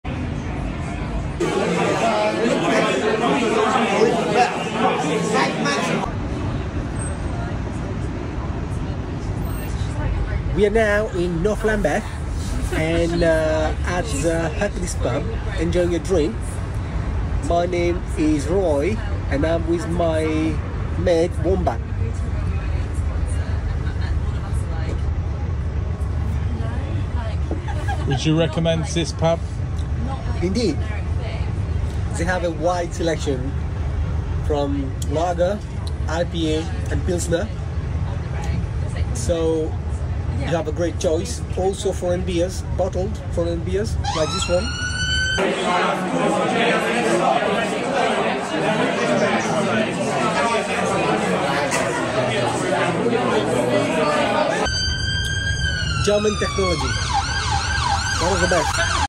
Hercules Pub - One of our Favourites in North Lambeth
In this Roy and I enjoy a beer in one of our favourite pubs in North Lambeth. This place specialises in craft beer. Lovely!